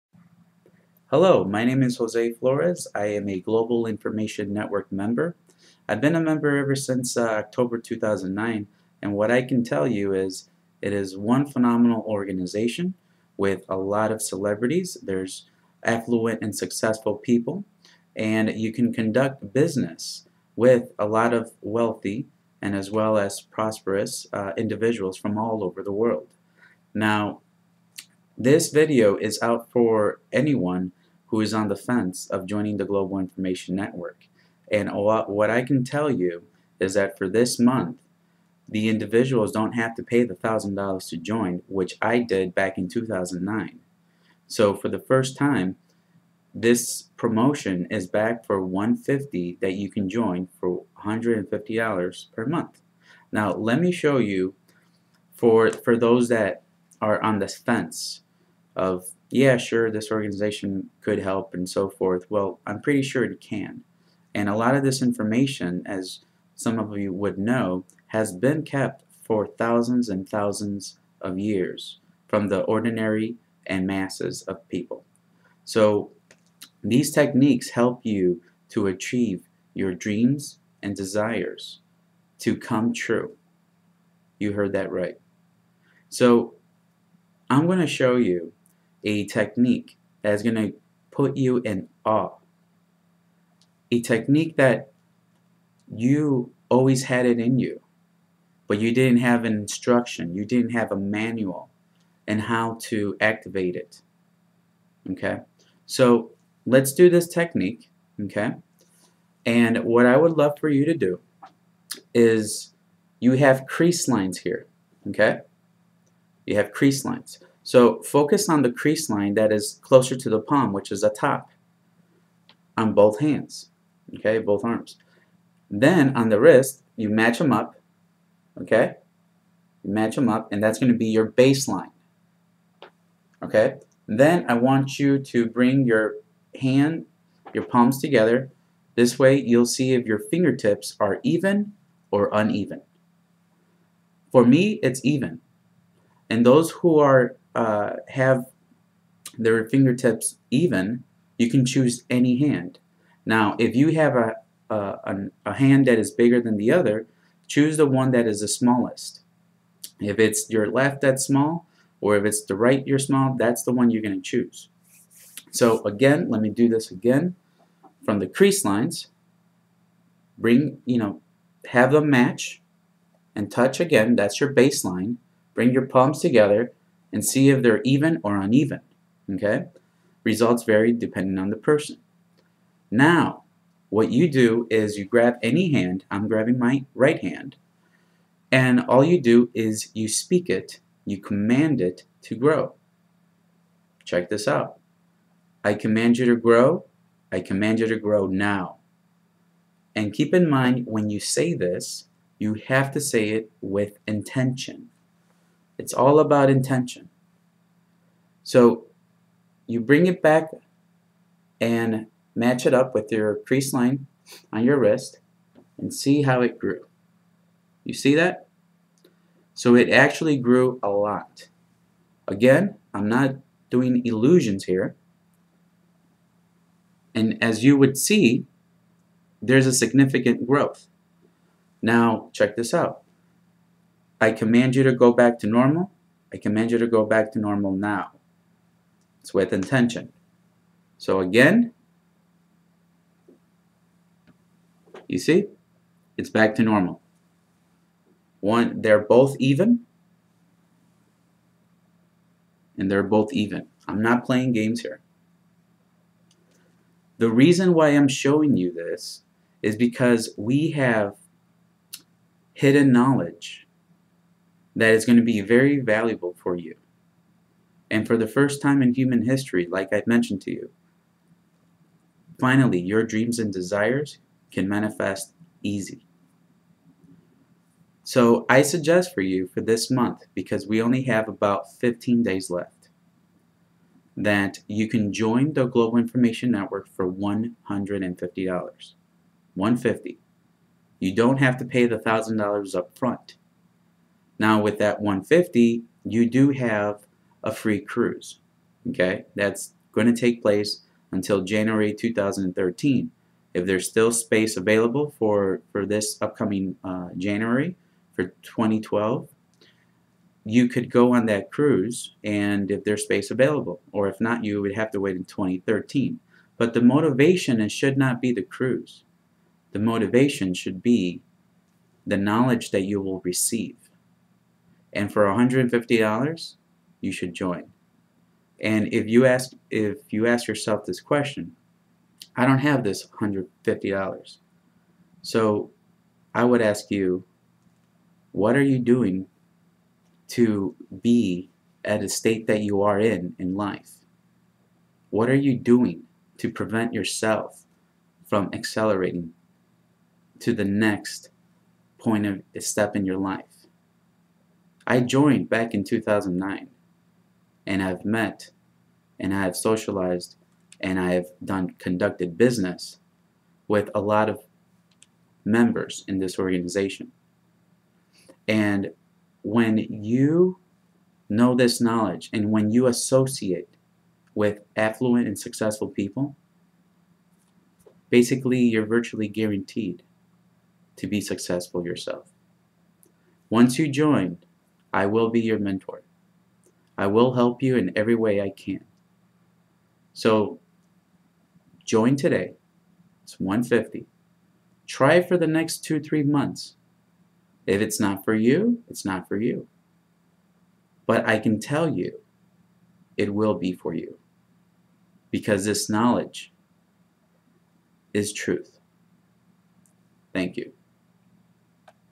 Global Information Network Member - Manifestation Technique
I will show you a visual technique on how your dreams and desires can come true. Watch this video if you want to learn how this technique works.